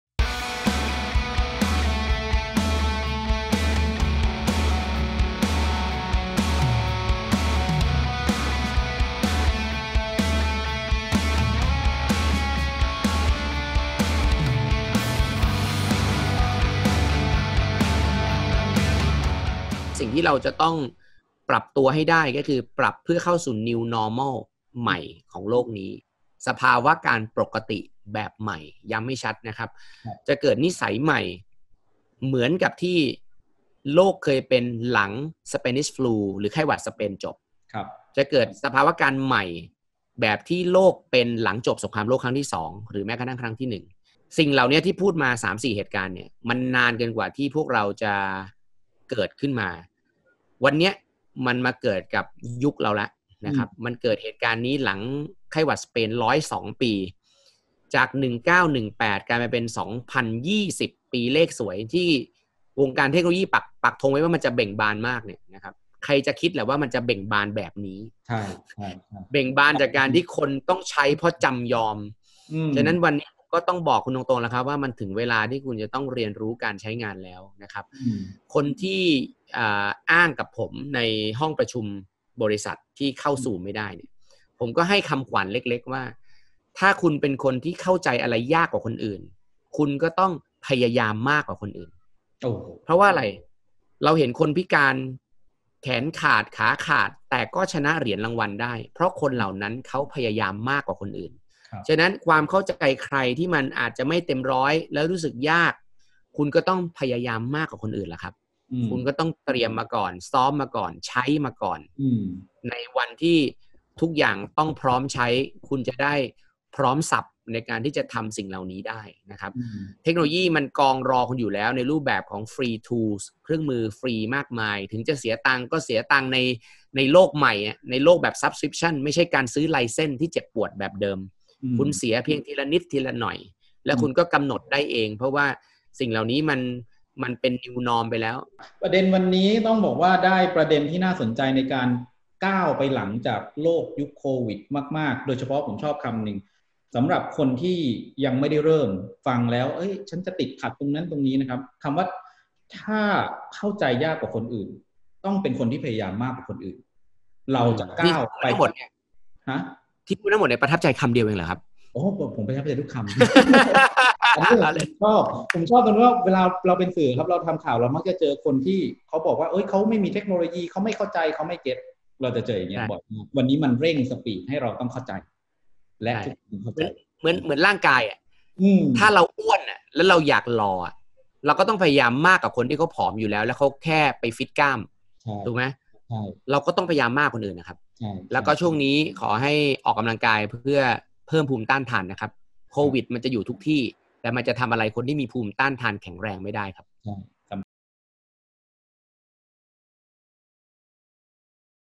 มนุษย์ยุคหลังโควิด-19 “ถ้าเข้าใจอะไรยาก ก็ต้องพยายามมาก | Springnews
หนุ่ย พงศ์สุข หิรัญพฤกษ์ แนะแนวทางการปรับตัว มุมมอง วิธีคิด เพื่อการอยู่ให้รอด ในโลกที่ไม่เหมือนเดิมอีกต่อไป โลกหลังยุคโควิด-19